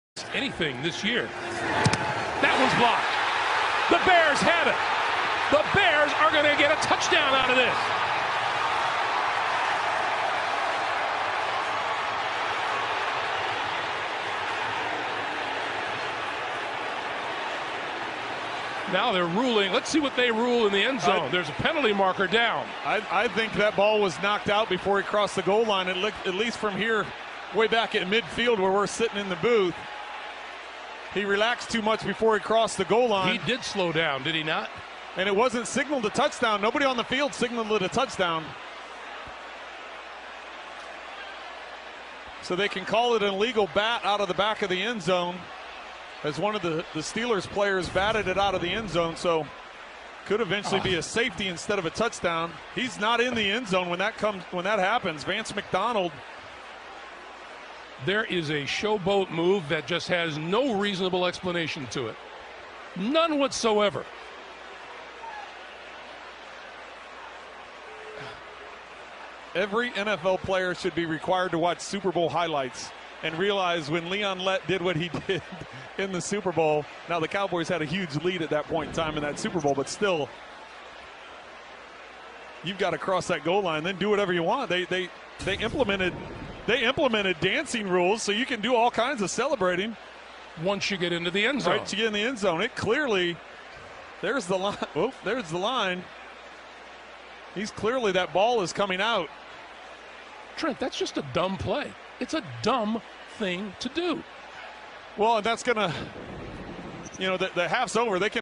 Marcus Cooper Sr. Costs Chicago Bears a Touchdown for Showboating Against Steelers!
As a Bears fan I can't say anything.what a dumbass. well done sr cooper. might even cause the bears the game.